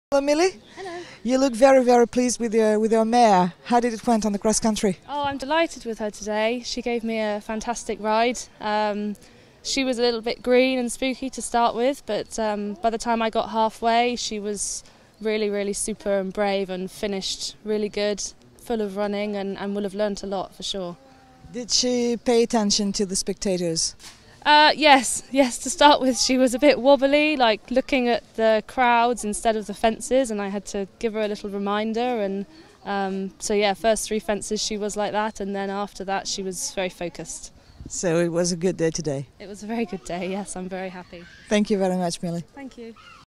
Mondial du Lion 2018: Camilla Millie DUMAS - UNIVERSAL COOLEY
Camilla Millie Dumas livre ses impressions à l'arrivée du cross du championnat du monde des 6 ans